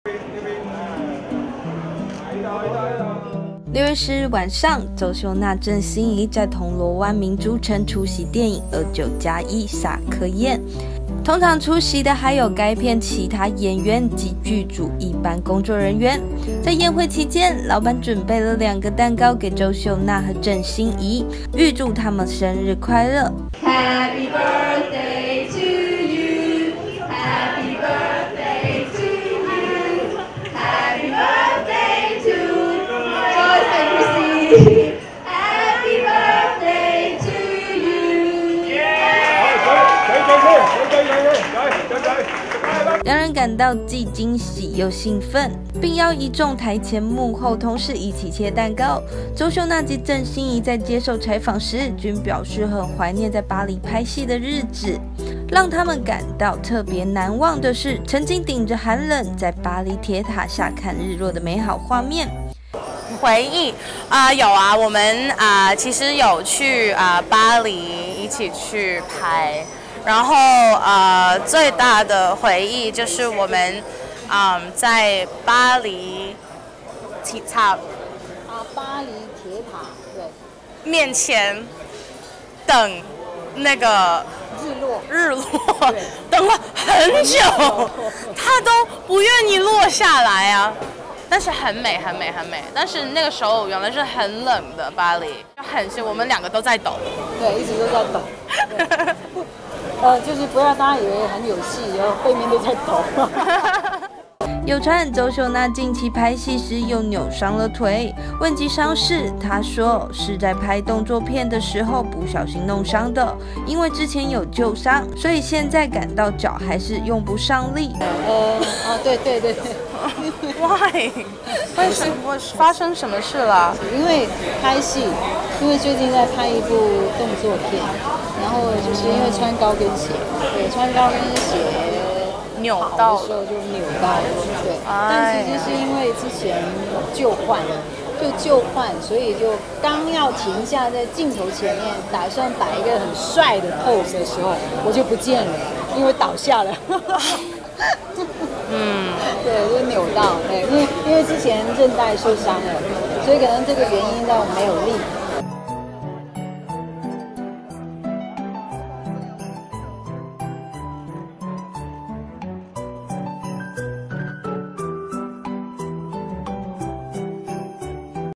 【4年前】周秀娜鄭欣宜煞科宴上慶生日 秀娜舊傷未愈又添新傷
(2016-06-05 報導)

6月4日晚上，周秀娜、鄭欣宜在銅鑼灣名珠城出席電影《29+1》煞科宴。同場出席的還有該影片其他演員及劇組一班工作人員，在宴會期間，老闆準備了兩個蛋糕送給周秀娜及鄭欣宜，預祝她們生日快樂。兩人感到既驚喜又興奮，并邀一眾臺前幕后同事一起切蛋糕。周秀娜及鄭欣宜在接受採訪時均表示很懷念在巴黎拍戲的日子，讓她們感到特別難忘的是曾經頂著寒冷在巴黎鐵塔下看日落的美好畫面。